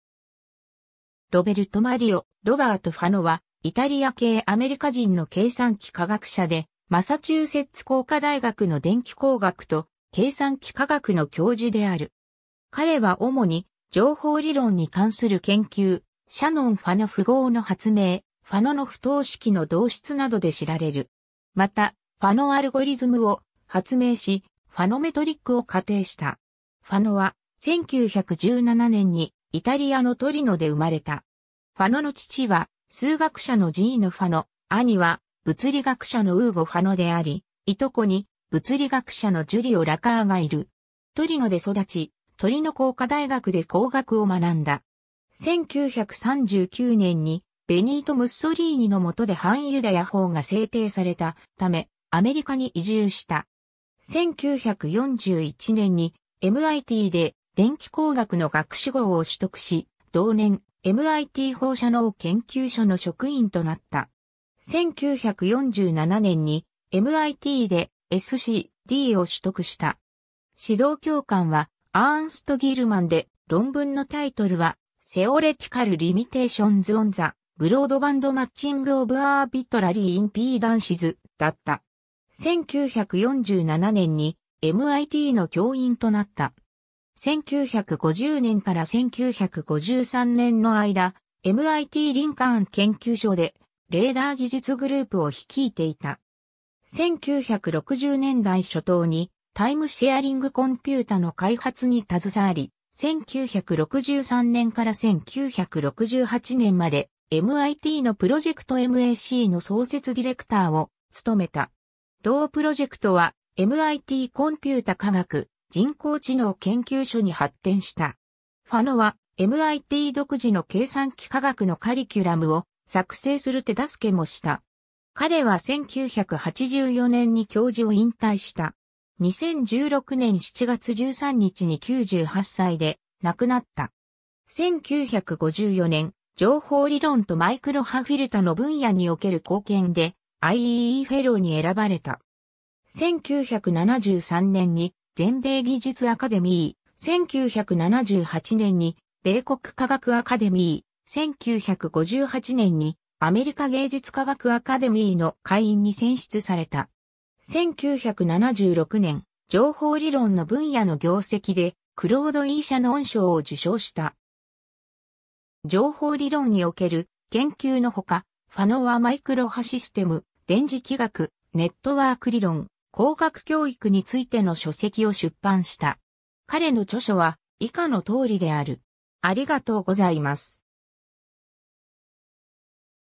ロバート・ファノ
ロベルト・マリオ・"ロバート"・ファノ（Roberto Mario "Robert" Fano、1917年11月11日 - 2016年7月13日）は、イタリア系アメリカ人の計算機科学者で、マサチューセッツ工科大学(MIT)の電気工学と計算機科学の教授である。
 彼は主に、情報理論に関する研究、シャノン・ファノ符号の発明（クロード・シャノンと共に）、ファノの不等式の導出などで知られる。
また、ファノアルゴリズムを発明し、ファノメトリックを仮定した。
 ファノは、1917年にイタリアのトリノで生まれた。
ファノの父は数学者のジーノ・ファノ、兄は物理学者のウーゴ・ファノであり、いとこに物理学者のジュリオ・ラカーがいる。
トリノで育ち、トリノ工科大学(Politecnico di Torino)で工学を学んだ。
1939年に、ベニート・ムッソリーニの下で反ユダヤ法が制定されたため、アメリカに移住した。
1941年にMITで電気工学の学士号を取得し、同年、MIT放射能研究所の職員となった。
1947年にMITでSc.Dを取得した。
指導教官はアーンスト・ギルマンで、論文のタイトルはTheoretical Limitations on the Broadband Matching of Arbitrary Impedances（任意インピーダンスの広帯域整合に関する理論的限界）だった。
1947年にMITの教員となった。
1950年から1953年の間、MITリンカーン研究所でレーダー技術グループを率いていた。
 1960年代初頭にタイムシェアリングコンピュータの開発に携わり、1963年から1968年までMITのProject MACの創設ディレクターを務めた。
同プロジェクトはMITコンピュータ科学・人工知能研究所に発展した。
ファノは、MIT独自の計算機科学のカリキュラムを作成する手助けもした。
彼は1984年に教授を引退した。
 2016年7月13日に98歳で亡くなった。
 1954年、「情報理論とマイクロ波フィルタの分野における貢献」でIEEEフェローに選ばれた。
 1973年に全米技術アカデミー、1978年に米国科学アカデミー、1958年にアメリカ芸術科学アカデミーの会員に選出された。
 1976年、情報理論の分野の業績でクロード・E・シャノン賞を受賞した。
 情報理論における研究のほか、ファノはマイクロ波システム、電磁気学、ネットワーク理論、工学教育についての書籍を出版した。
彼の著書は以下の通りである。